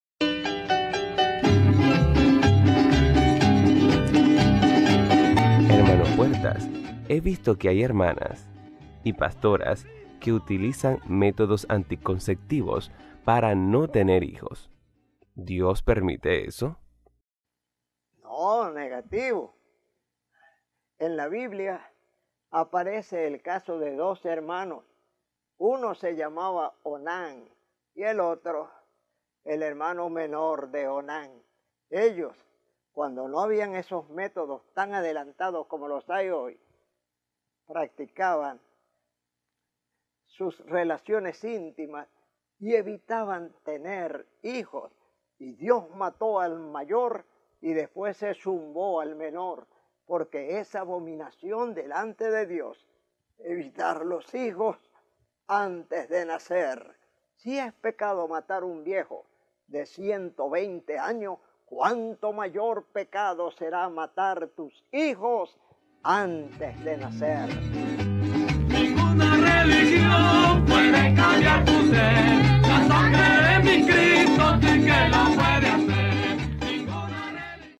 HNO. PUERTAS RESPONDE #238 | ¿QUE OPINA DE LOS MÉTODOS ANTICONCEPTIVO | LUZ TV | MICRO HNO PUERTAS
Hno. Puertas; He visto que hay hermanas y pastora que utilizan métodos anticonceptivos para no tener hijos, ¿Dios permite eso?